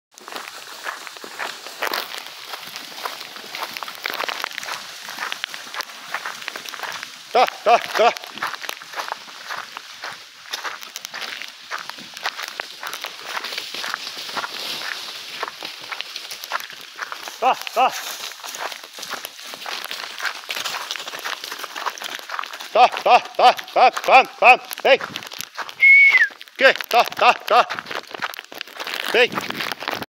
20210827 161328 estouro da boiada
Bois bem loucos pra chegarem na cocheira.
Não tem trabalho mais gratificante que o trabalho digno